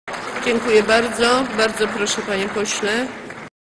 [148/276] Ewa Kopacz: Dziękuję bardzo. Bardzo proszę, panie......
Posiedzenie Sejmu nr 5
13 stycznia 2012 r. 12:43:34
Marszałek Ewa Kopacz:

Dziękuję bardzo.  Bardzo proszę, panie pośle.